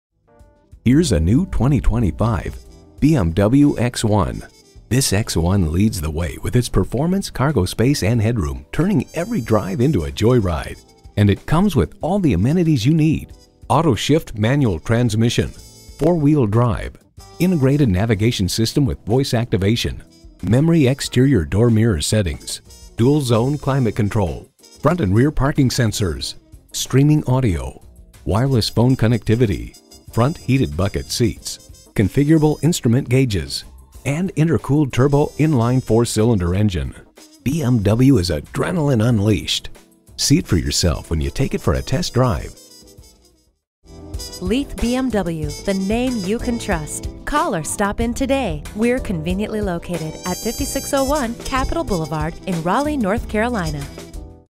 New 2025 BMW X1 Raleigh For-Sale, NC
Year: 2025
Make: BMW
Model: X1
Trim: xDrive28i
Engine: 2L I-4 port/direct injection, DOHC, variable valve control, intercooled turbo, premium unleaded, engine with 241HP
Transmission: Automatic
Color: Black Sapphire Metallic
Interior: Oyster
Stock #: W502549
VIN: WBX73EF09S5122357

Since 1969 Leith Inc. Has Been The Name you Can Trust In the Raleigh Durham Area.

Address:
5601 Capital Blvd